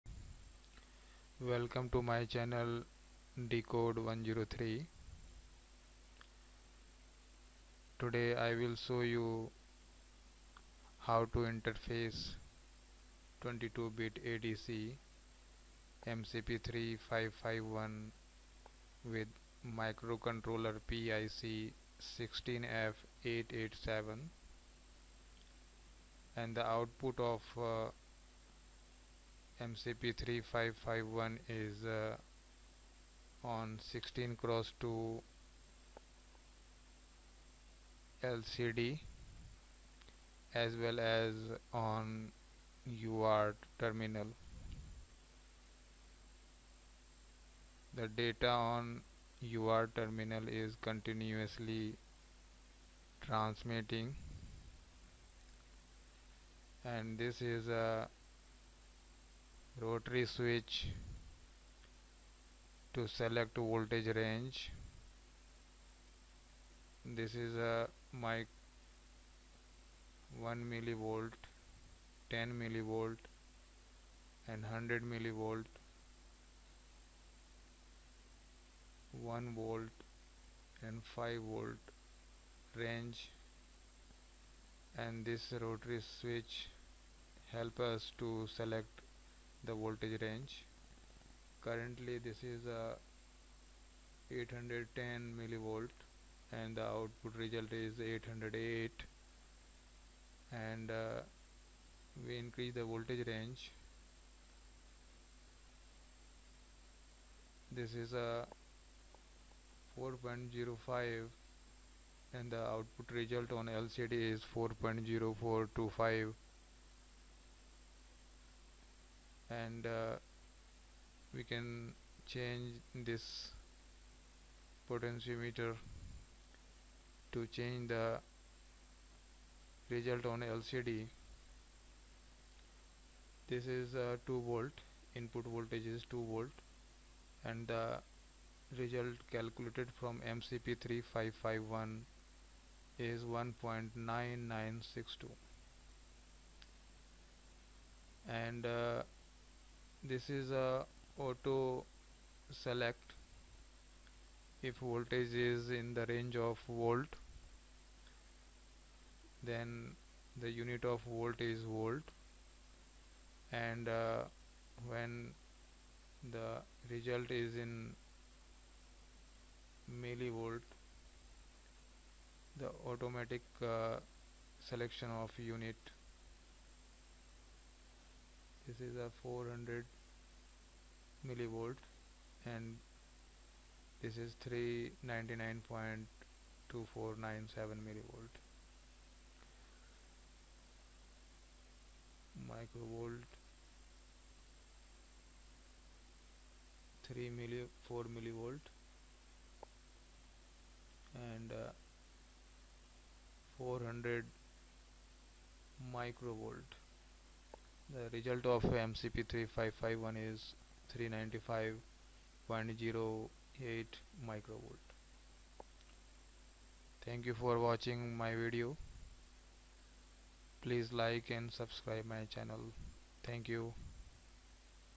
Automatic DC voltmeter | PIC16F886 | MCP3551 | 16X2 LCD
Welcome to Dasjee project solutions

VOLTMETER using PIC16F886 AND MCP3551, 16X2 LCD

MCP3551 is 22 bits ADC

Voltage range 0 to 5V